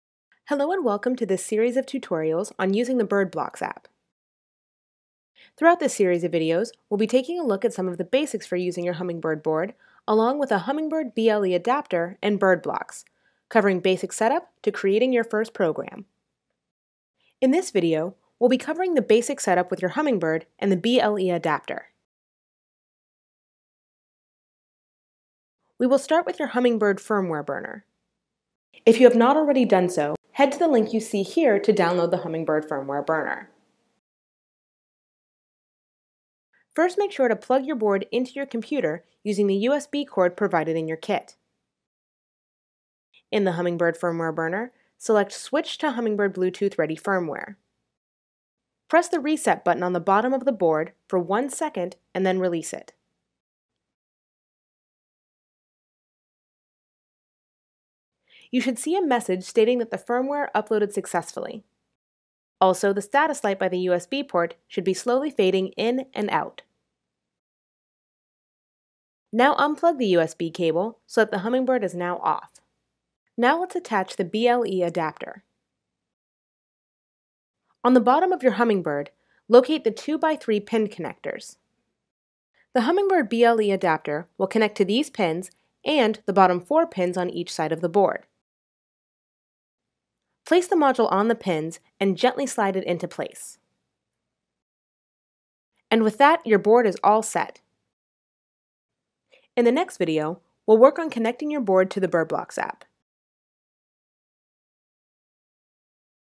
Using BirdBlox: Setting Up Your Board
In this series of videos we take you through using the BirdBlox app and BLE adapter. This first video gives you a look at to set up your Hummingbird board for BirdBlox.

More info...